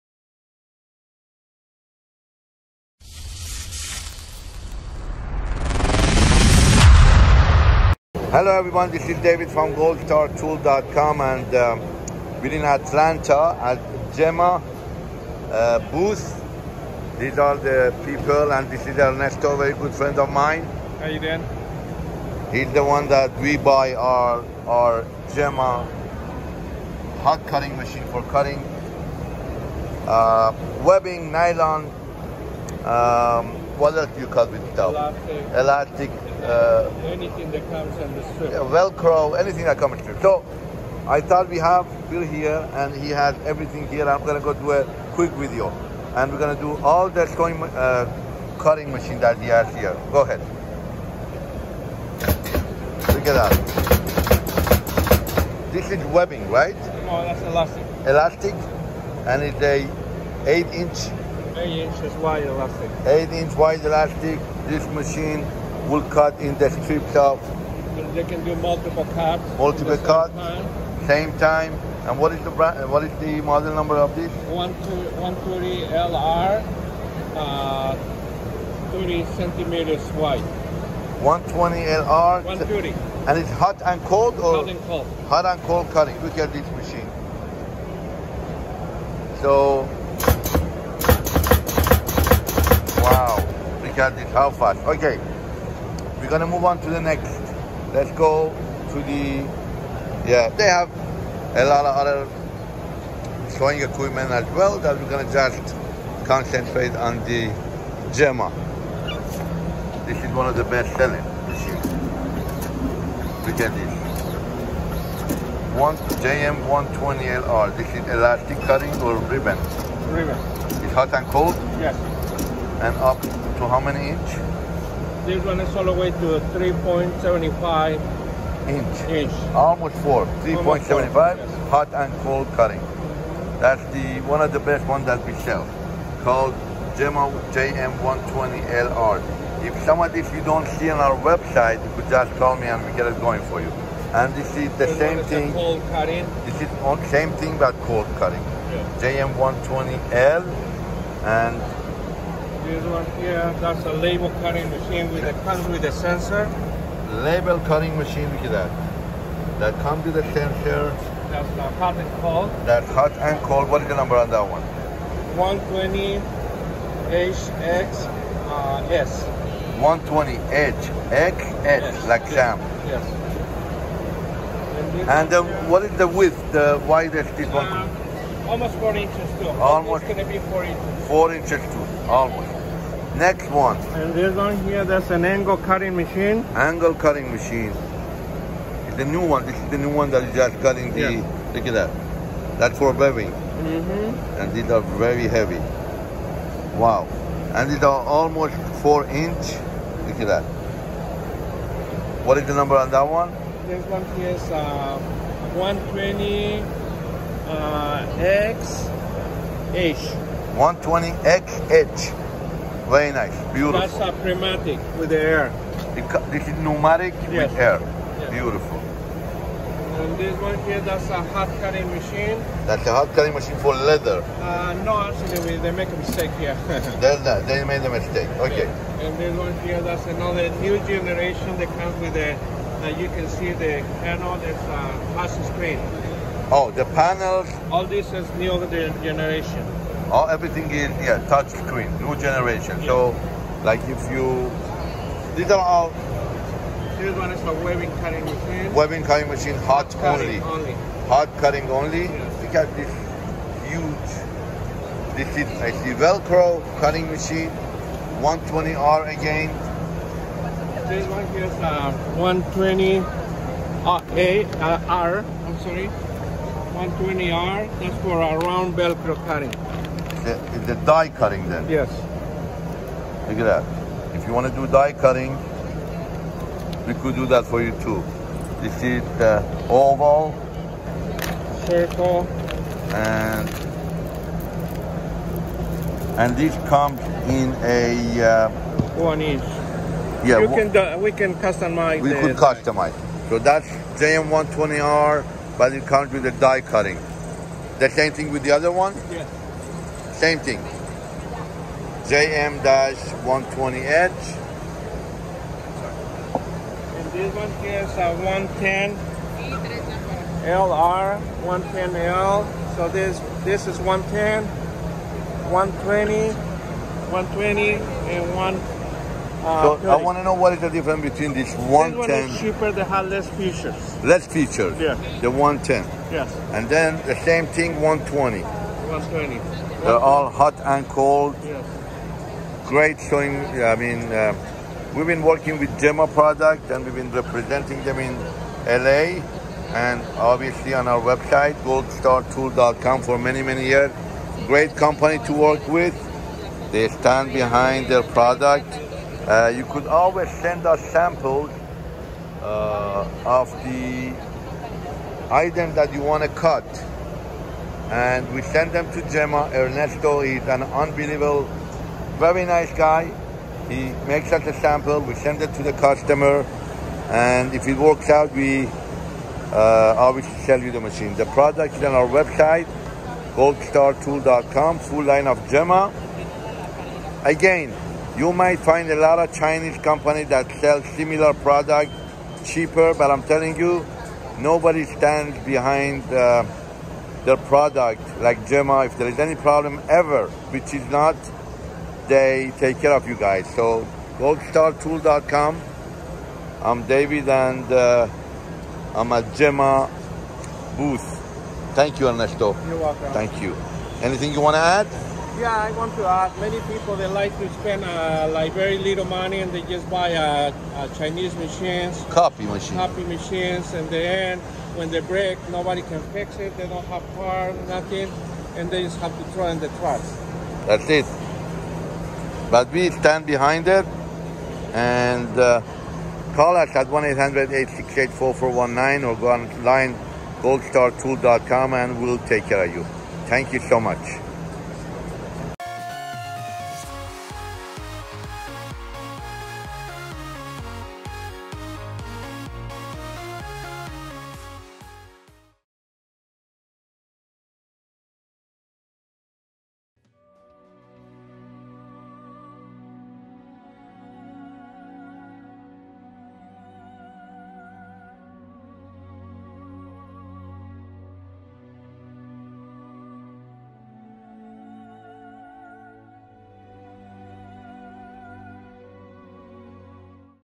Trade Show - JEMA Heavy Duty Cutting Machines - Goldstartool.com
In today's video, David continues his tour of the Atlanta trade show, visiting the JEMA booth to take a look at their heavy duty cutting machines.